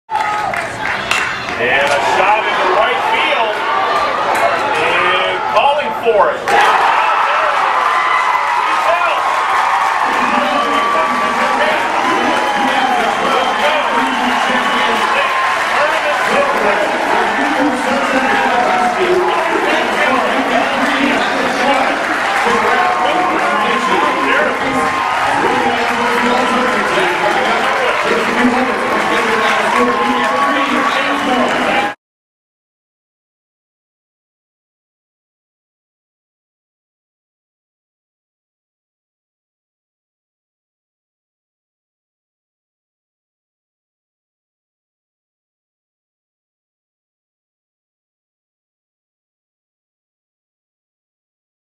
Dom Pangallo's diving catch - NewCath wins 9th Region
for the best NKy high school sports coverage.